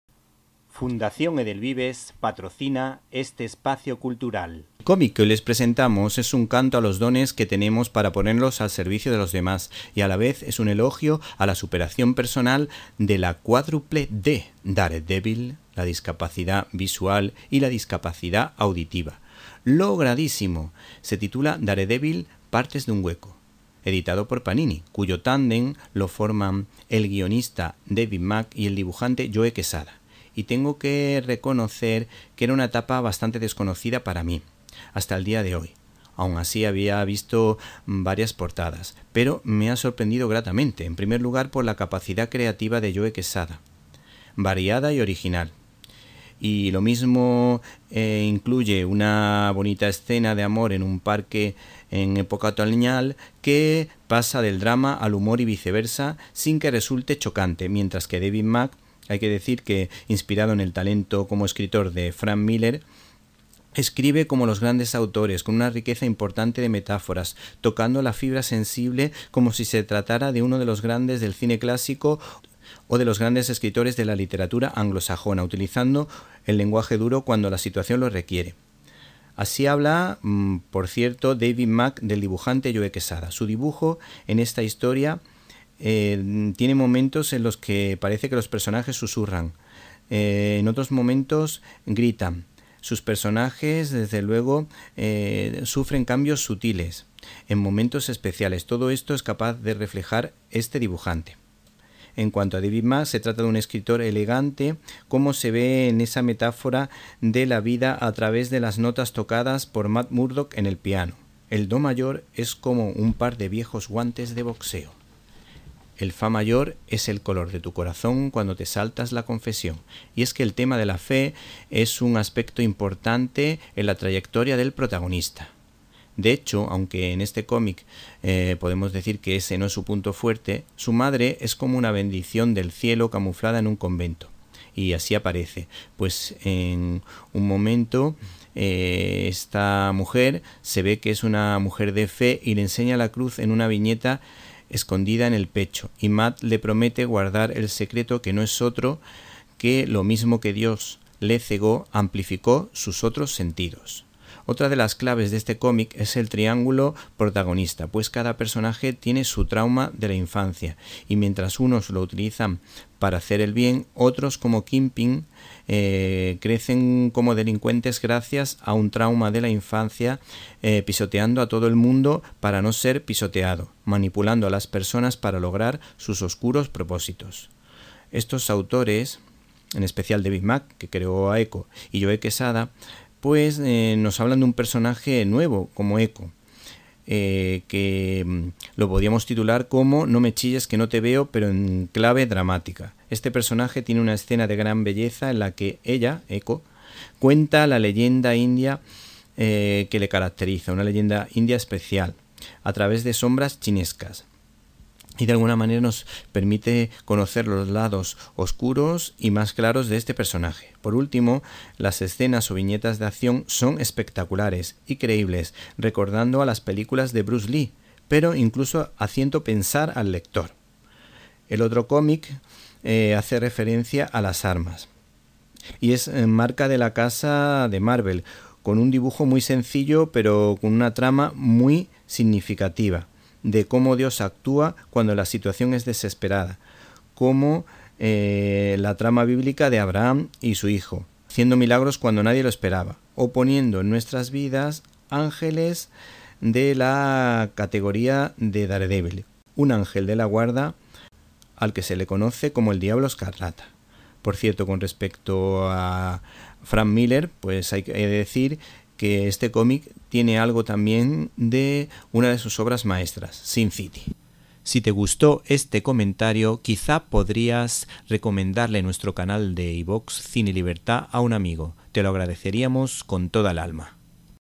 Daredevil: Partes de un hueco-La música y la Providencia bajo la batuta de Joe Quesada y David Mack
(selección de contenido) Daredevil: Partes de un hueco-La música y la Providencia bajo la batuta de Joe Quesada y David Mack. ¿Qué virtudes encontramos en este providencial tebeo? ¿Dios está cerca de los más débiles?

Aquí puedes escuchar mi comentario...

¡Lectores y amigos de Cine y Libertad!

Esta es la sinopsis oficial de Daredevil: Partes de un hueco-La música y la Providencia bajo la batuta de Joe Quesada y David Mack:

Ella se llama Eco, y aunque parece el alma gemela de Daredevil, podría convertirse en su peor enemiga. Maya busca al asesino de su padre, y está convencido que no es otro que Matt Murdock. El aclamado ilustrador David Mack se une a Joe Quesada en la ambiciosa secuela de “Diablo Guardián.

Víctor Alvarado